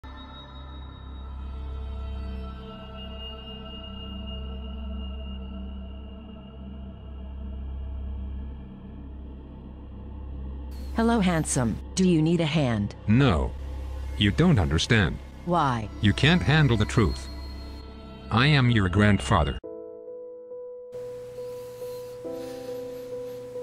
a very "handy" film
in this video, i teach how you to make a film without friends with your hands.